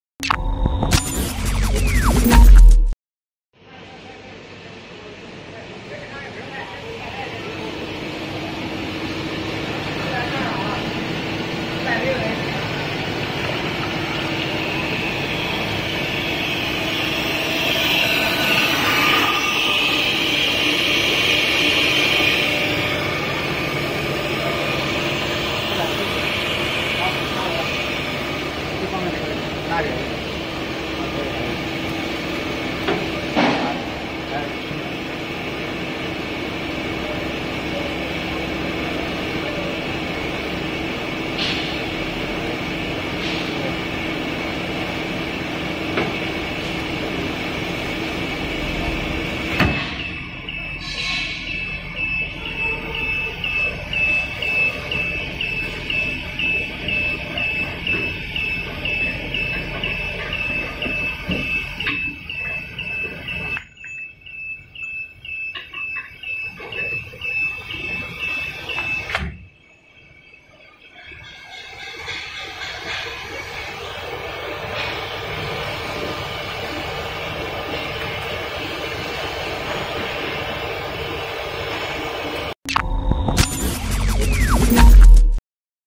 3D filament machine with automatical cutting system
3D printer filament making machine testing...
1. Automatically cut the filament once one spool is finished.
2. 15~30KG/H capacity
3. Ready to ship to Australia.